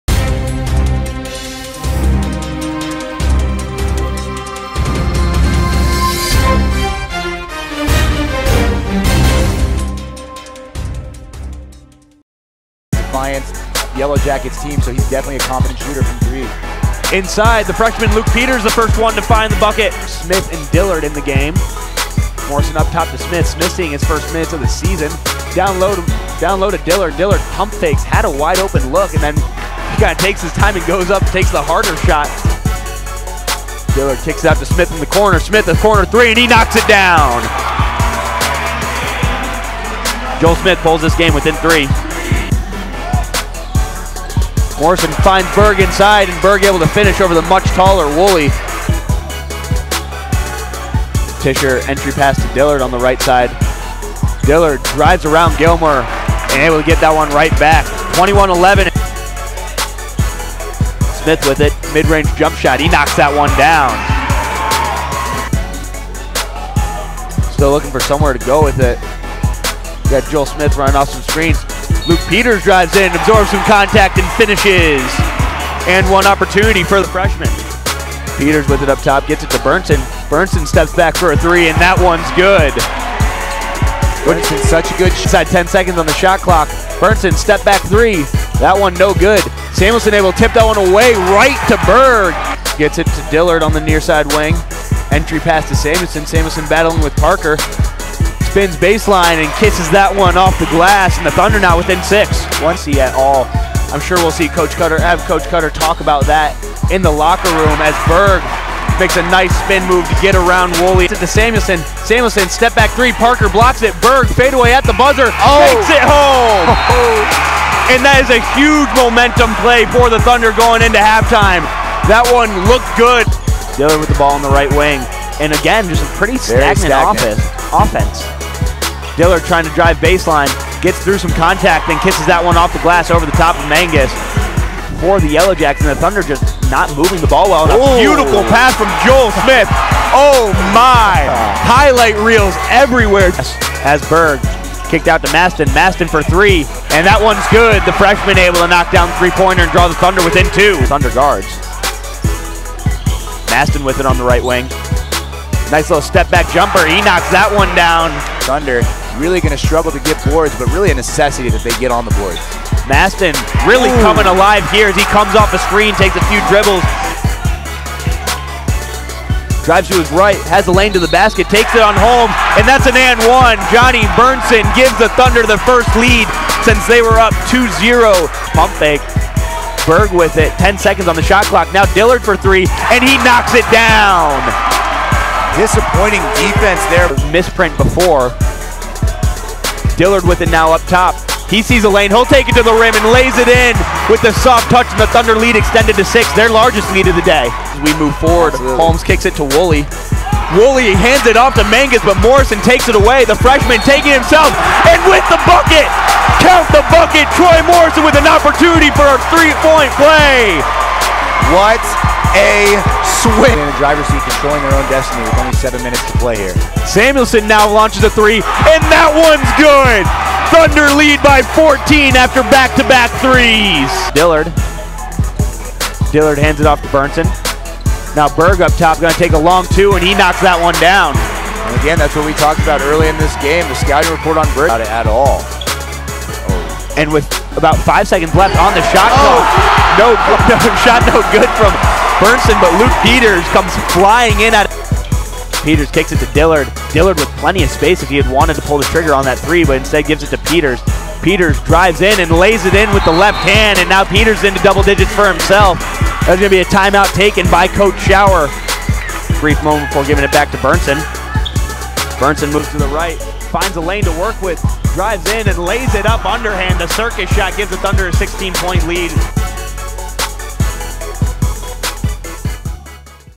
Wheaton - Defiance MBB Lee Pfund Tournament Highlights
Highlights from Wheaton's 76-61 victory over Defiance College on 21 Nov 2015 in the Lee Pfund Classic Tournament.